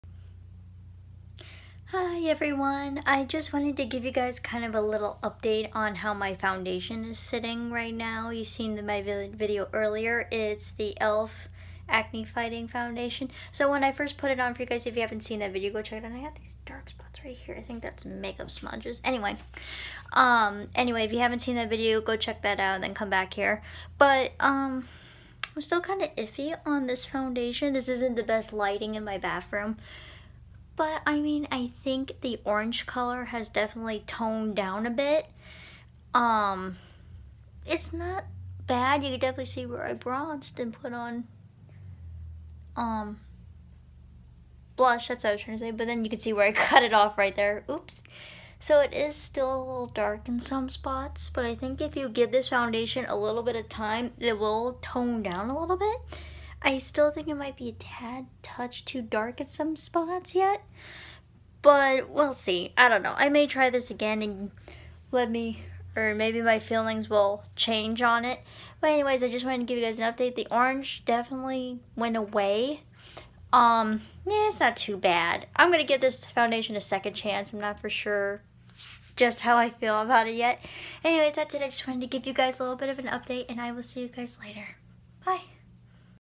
An update on the e.l.f. Acne fighting foundation
It’s been a couple of hours of wear time and hears what I think about it now